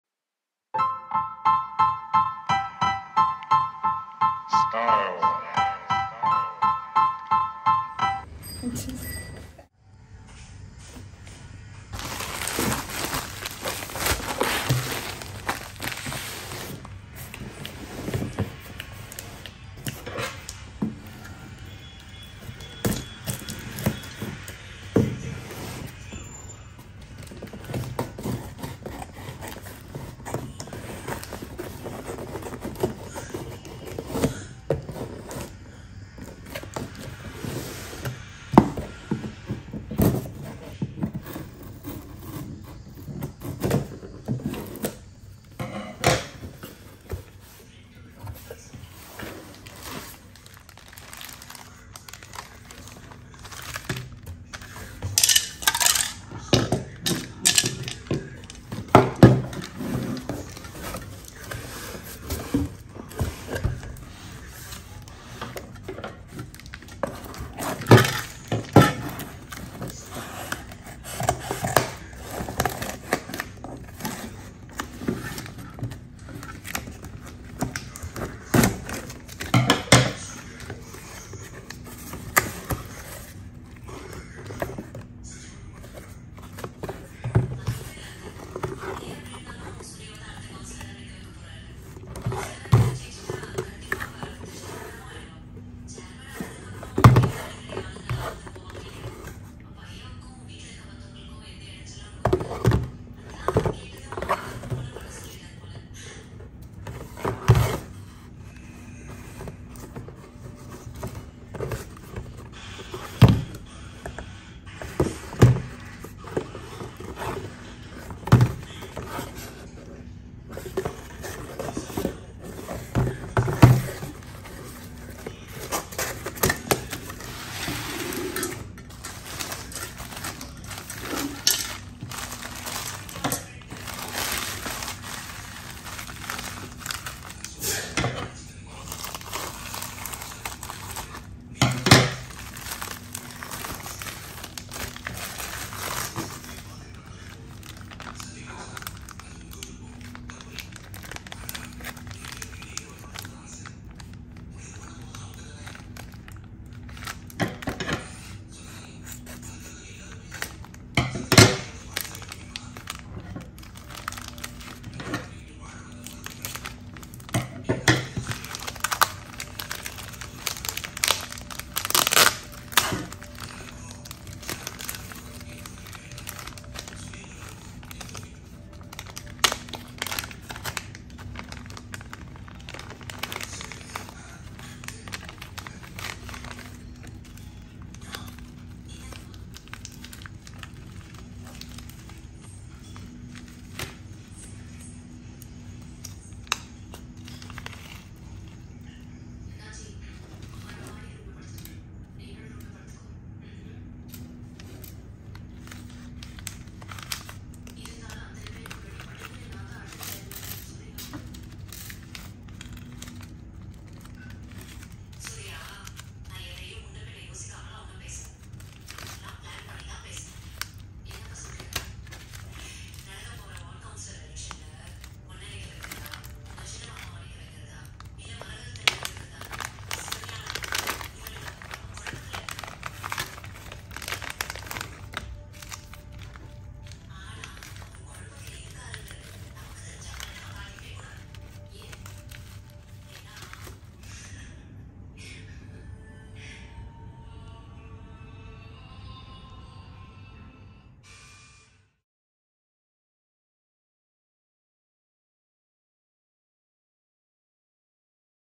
Unboxing Art Supplies | @anbuartcreations@amazon
Hello Everyone,

In this video i will be unboxing my new art supplies that i bought from Amazon, Hope you guys will like this video! Keep creating!